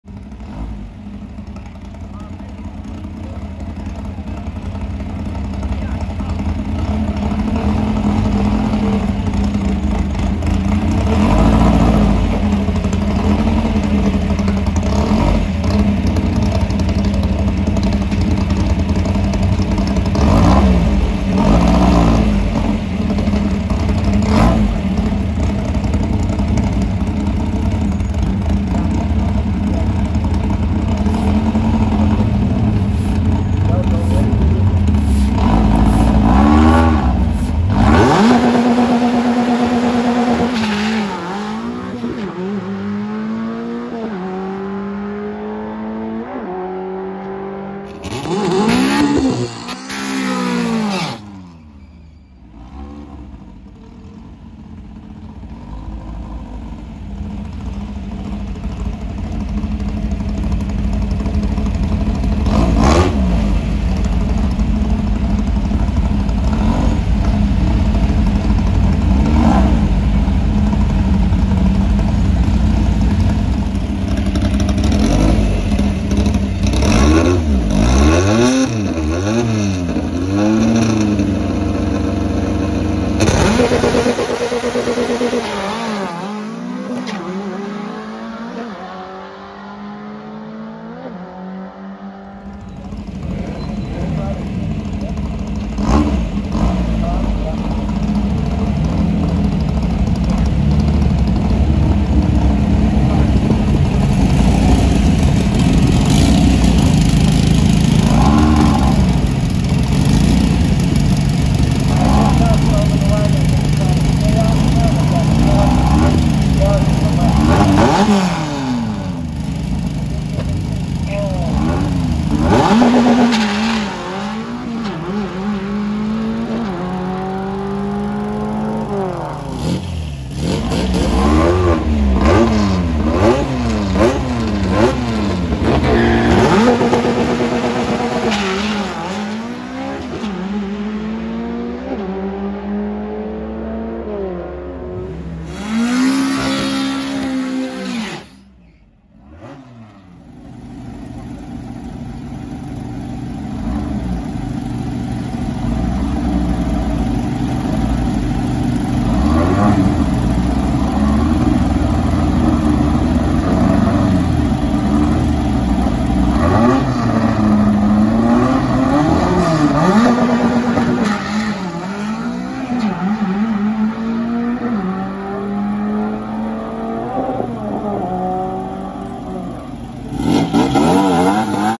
Powerhaus VW Drag Bug Making its First Few Passes On The Track
Powerhaus Drag Bug
Frameworks Tube Chassis
Autocraft 3054cc Redline Weber Fuel Injected 237whp Naturally Aspirated
Autocraft swing axle transmission
@AldanAmerican coil over suspension
1/8 mile et  6.63 @101mph